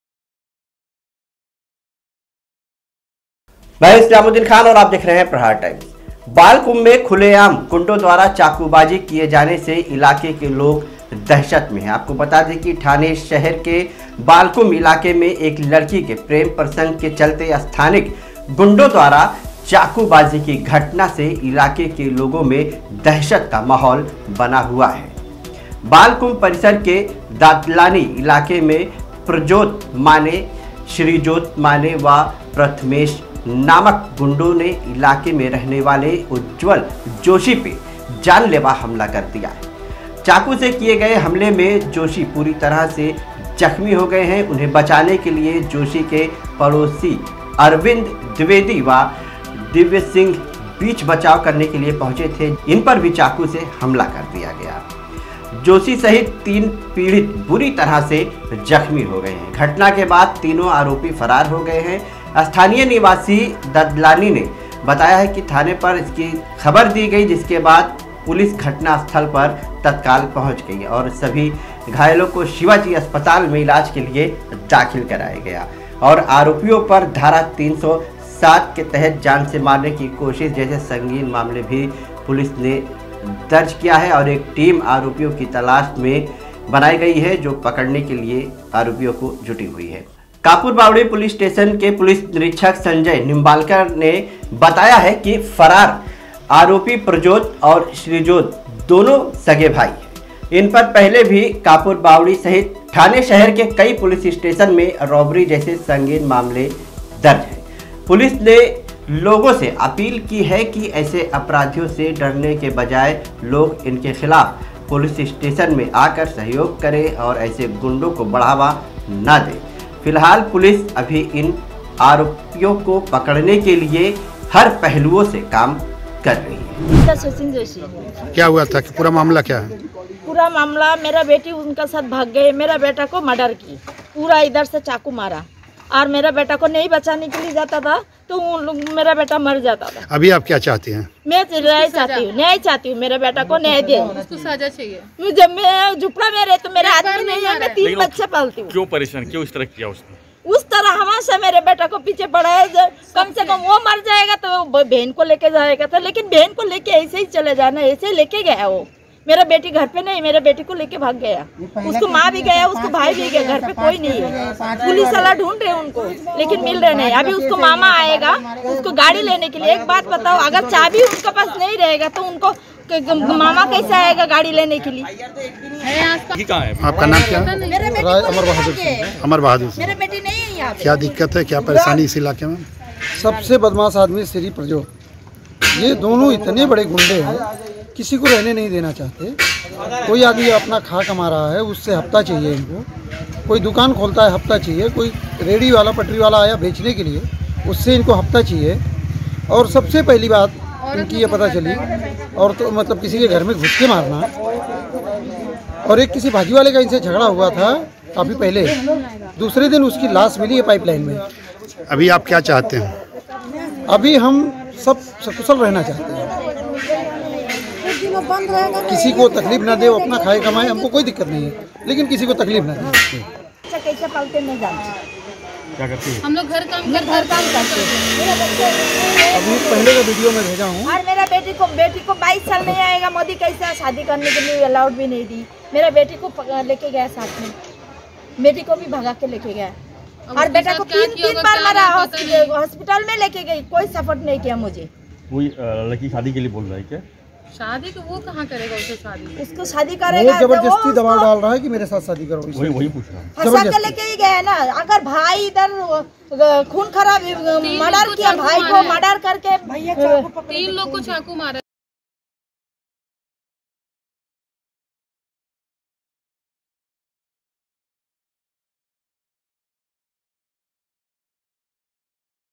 प्यार के चक्कर मे जानलेवा हमला || Islamuddin Khan || Aapka Prahar Times
प्यार के चक्कर मे जानलेवा हमला || Islamuddin Khan

थाने शहर के बालकुम इलाके मे एक लड़की के प्रेम प्रसंग के चलते स्थानिक गुंडों द्वारा चाक़ू बाजी की घटना से इलाके के लोग दहशत के साये मे जिने को मजबूर हैँ बालकुम परिसर के दादलानी इलाके मे प्रजोत माने, श्रीजोत माने व प्राथमेश नामक गुंडों ने इलाके मे रहने वाले उज्जवल जोशी पे जानलेवा हमला कर दिया, चाकू से किये गये हमले मे जोशी बुरी तरह से जख्मी हो गये उन्हे बचाने के लिए जोशी के पड़ोसी अरविंद द्विवेदी व दिव्या सिंह बिच बचाव करने पहुंचे जिन्हे भी चाकू से हमला किया गया उसमे जोशी सहित तीनो पीड़ित बुरी तरह से जख्मी हो गये, घटना के बाद तीनो आरोपी फरार हो गये।
 स्थानीय निवासी, दादलानी, ने थाने पर खबर दी खबर मिलते ही पुलिस घटनास्थल पर पहुँच कर सभी घायलों को शिवाजी अस्पताल दाखिल किया, और आरोपियों पर धारा 307 जान से मारने की कोसिस जैसे संगीन मामला दर्ज कर पुलिस ने एक टीम बना के आरोपियों की तलाश मे पुलिस लगी हुई है

कापुर बावड़ी पुलिस निरीक्षक संजय निम्बालकर कापुर ने बताया कि
फरार आरोपी प्रजोत और श्रीजोत दोनो सगे भाई हैँ इन पर पहले भी कापुर बावड़ी सहित थाने शहर् के कई पुलिस थाने मे रोबरी जैसे संगीन मामले दर्ज हैँ पुलिस ने लोगों से अपील की है की ऐसे अपराधियों से डरने की बजाय लोग इनके खिलाफ पुलिस थाने आकर सहयोग करें और ऐसे गुंडों को बढ़ावा ना दें फिलहाल पुलिस अभी इन अपराधियों को पकड़ने के लिए हर पहलुओं पे काम कर रही है